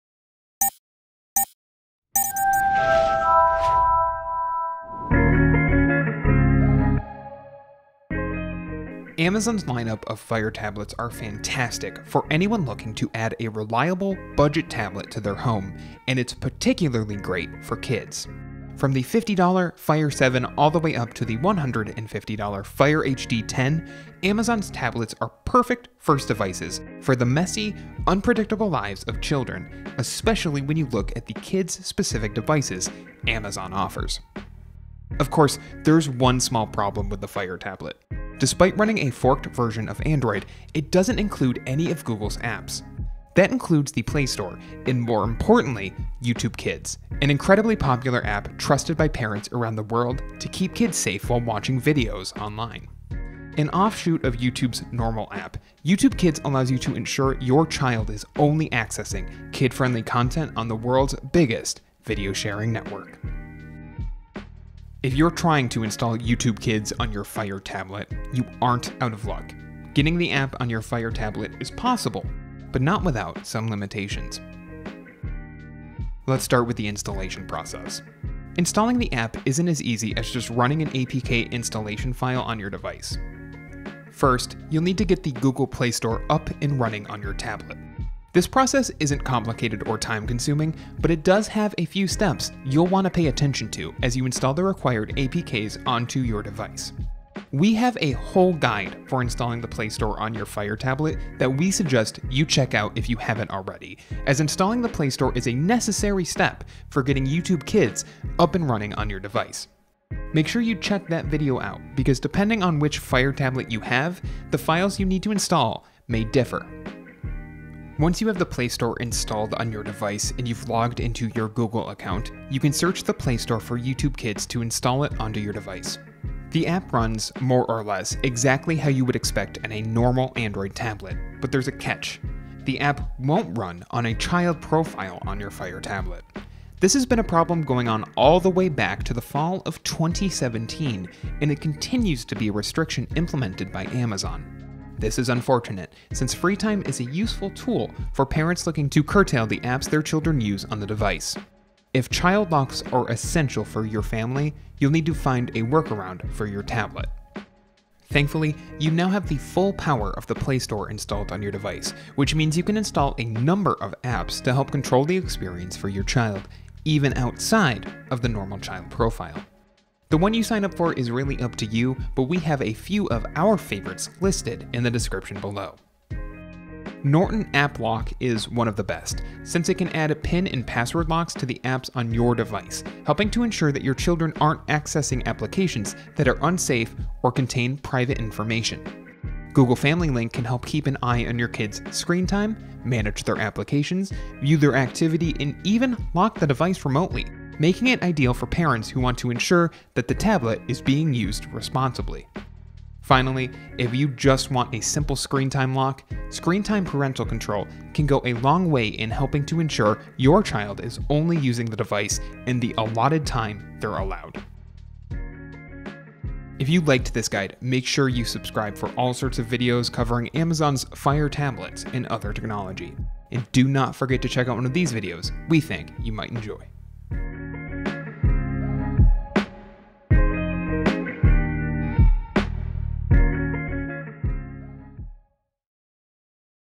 How to INSTALL YouTube Kids on a Fire Tablet! [2020 Update!]
Getting YouTube Kids installed on your Fire Tablet is a snap, but getting it to work on a kid-friendly tablet is a bit more difficult. Our guide walks you through your options.